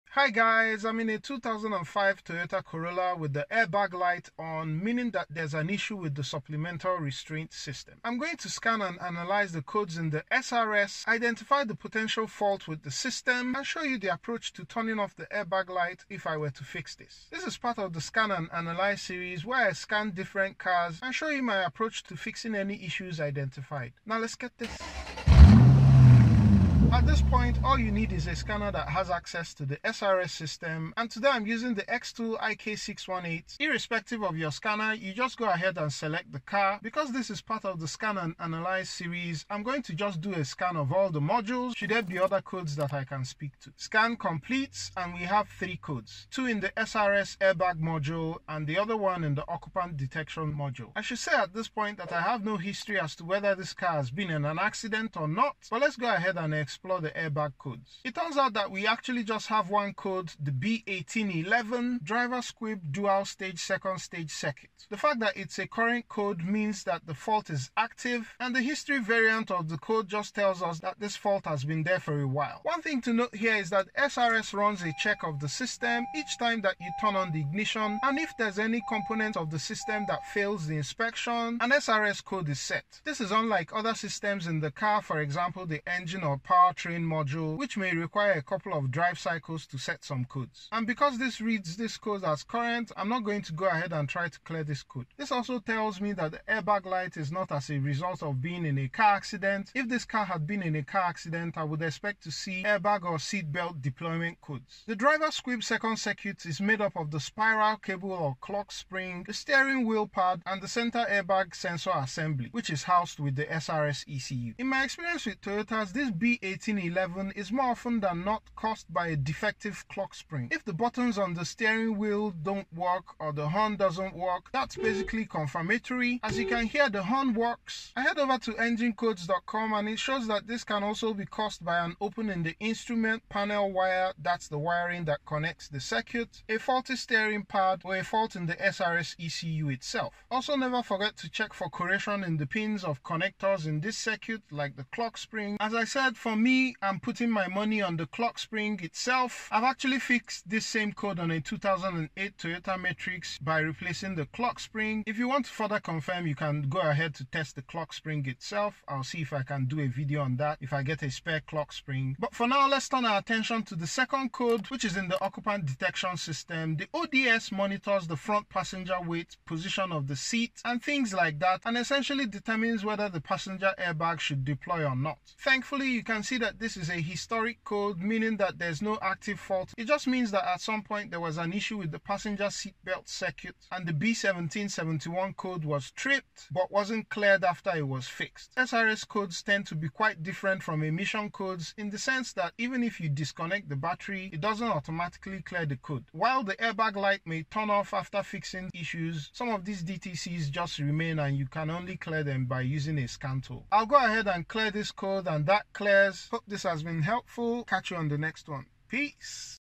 Scan and Analyze: Toyota Airbag Light Troubleshoot / Why the Airbag Light is ON: Scanning SRS module
I scan and analyze this Toyota airbag light and get to why the airbag light is on. Scanning SRS module is vital if the SRS light stays on. This is part of a new scan and analyze series on the channel. This series will cover my approach to fixing issues identified while scanning and analyzing diagnostic trouble codes in cars. If your Toyota airbag light stays on, you may find this video useful. Although this video deals with the Toyota Corolla airbag light, it will apply to other Toyota e.g., Camry, Rav4 etc. SRS light stays on

Timestamps:
0:00 - Intro
0:26 - Scanning the car
0:43 - Scan results overview
0:55 - Analysis & cause - Airbag codes
2:42 - Analysis & cause - ODS codes